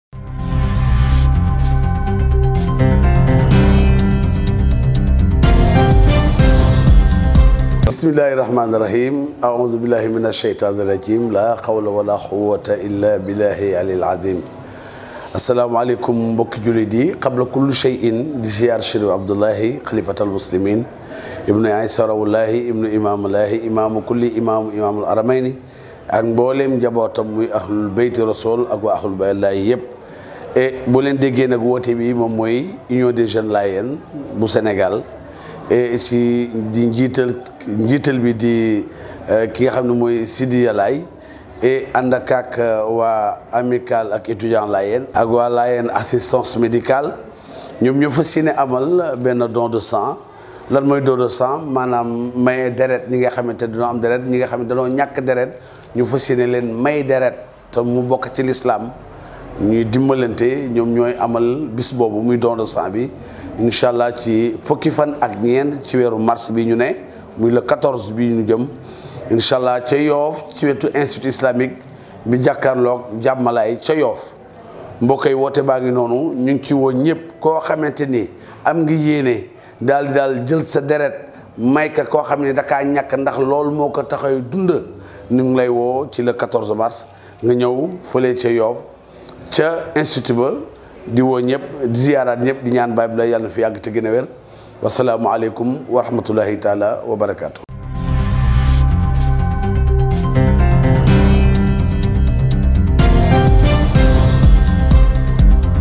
Don De Sang. Union Des Jeunes Layénes: Message de Ibrahima Diongue Laye (Gazou Laye).
DON DE SANG ORGANISE PAR UNION DES JEUNES LAYENES. Ce Samedi 14 Mars 2020 à Yoff Layénes. A cet occasion Union des jeunes layénes rend hommage à ILLIMANE SENY DIAGNE et PAPE ASSANE DIENE.
VOTRE PRÉSENCE EST VIVEMENT SOUHAITÉE.